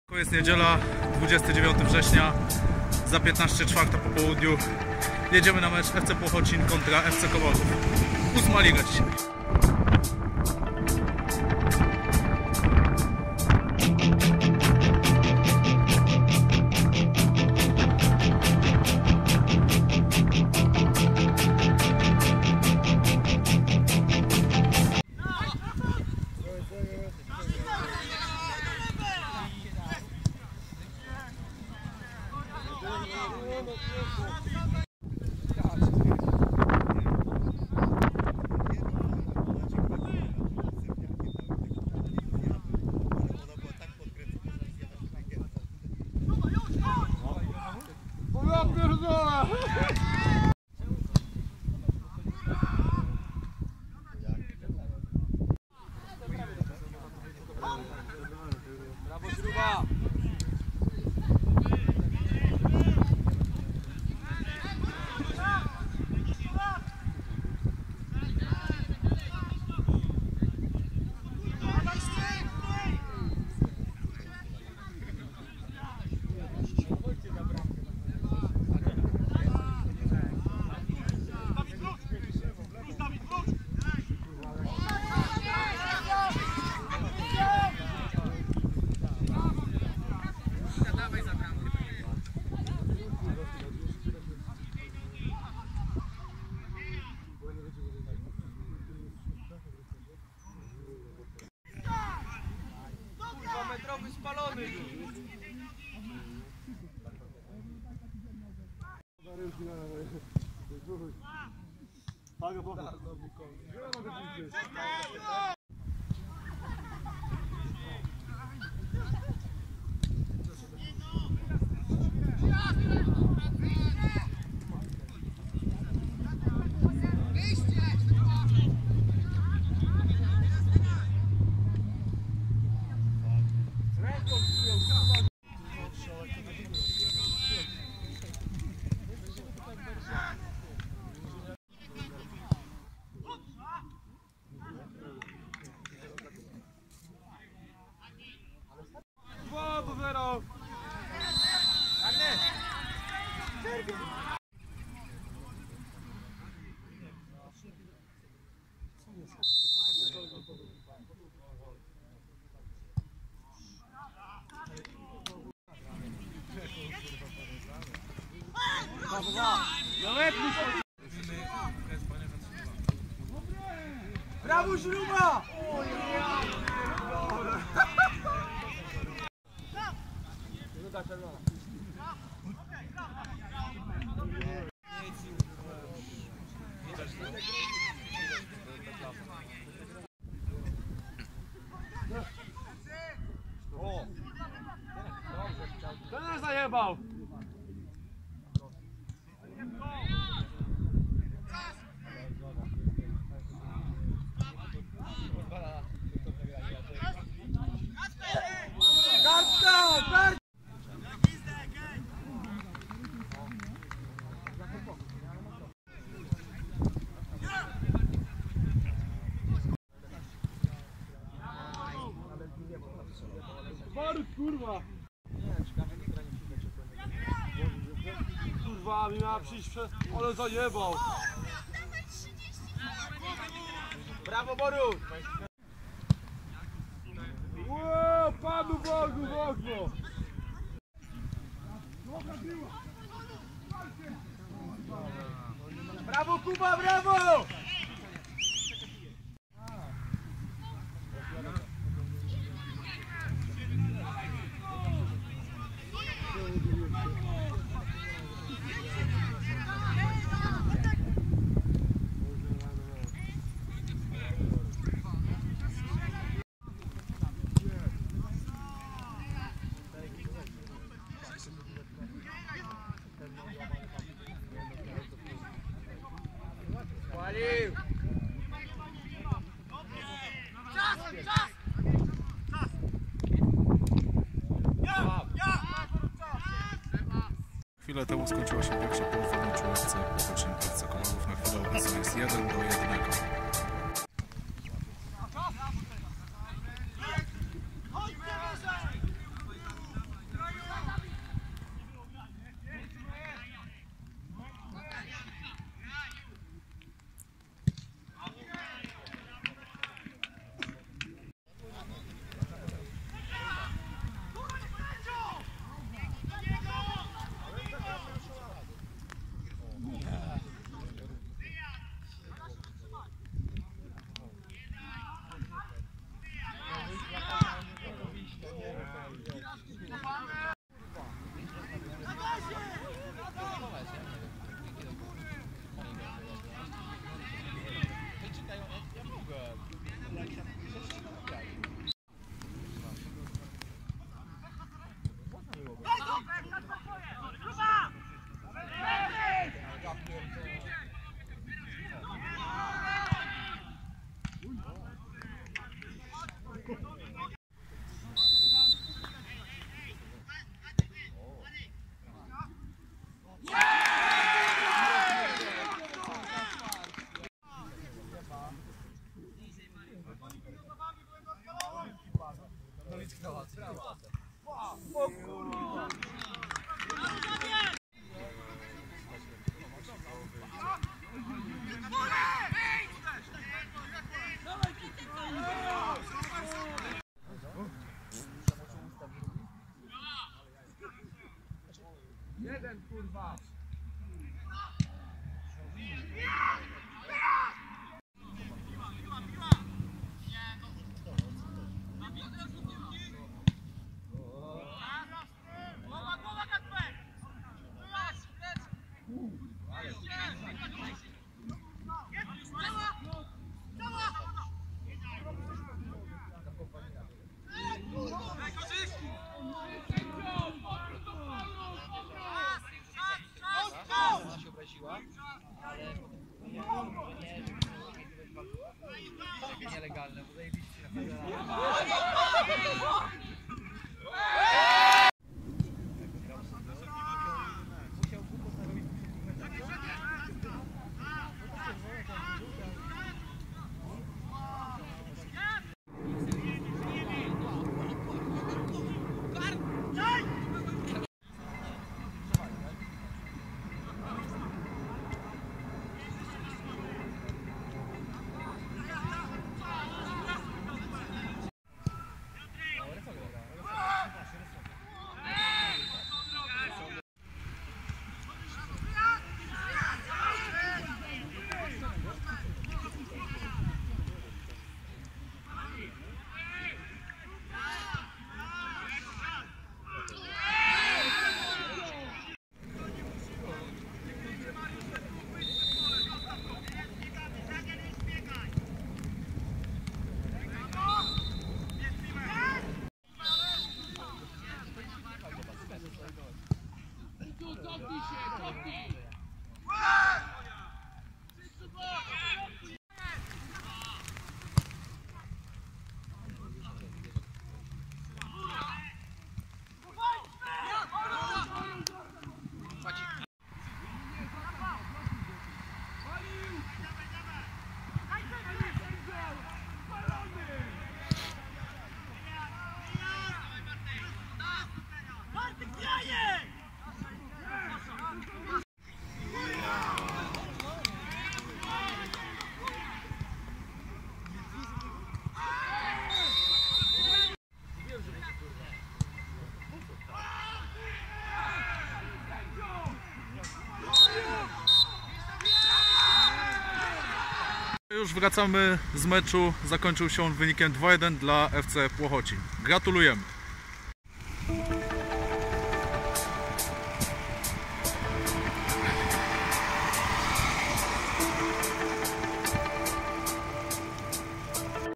Meczowo#30 FC Płochocin vs. FC Komorów (29.09.2019 r.)
- Dzień meczu i godzina: 29 września 2019, godz. 16:00
- Rozgrywki: B-klasa gr. Warszawa IV, 7. kolejka
- Drużyny: FC Płochocin - FC Komorów
- Miejsce rozgrywanego meczu: boisko sportowe w Płochocinie (ul. Lipowa 3, Płochocin)
- Wynik: 2:1
- Widzów: +/- 35